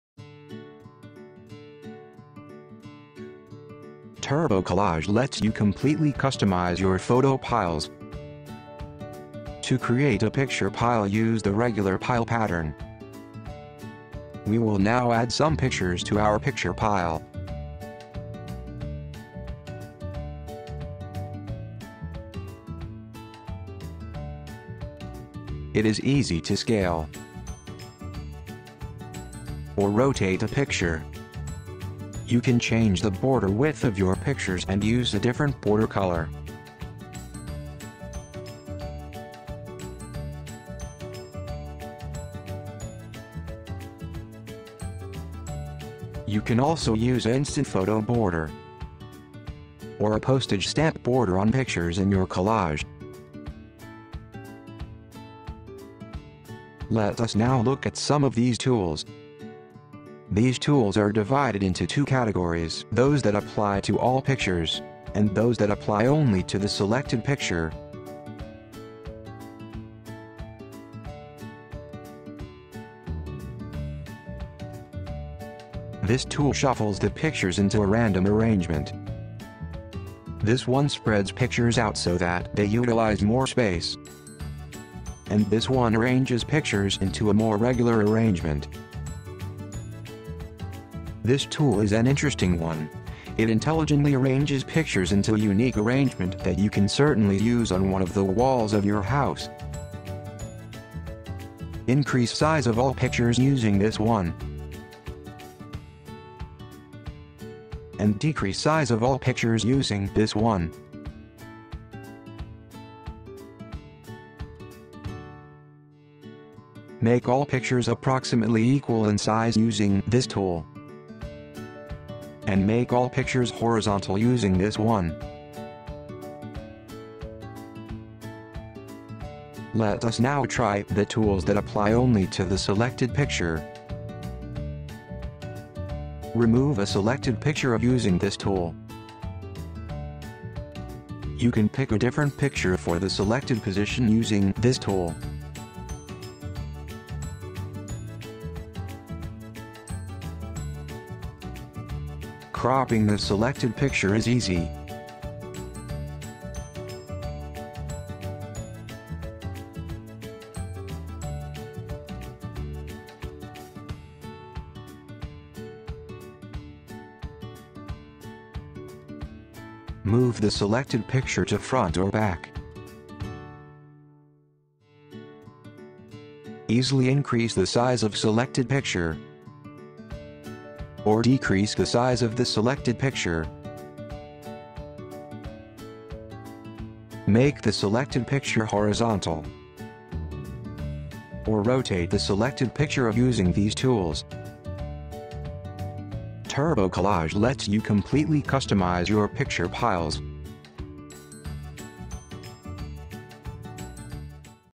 How to Create Photo Piles | TurboCollage
Learn to make a freeform pile collage from photos on your Mac or PC!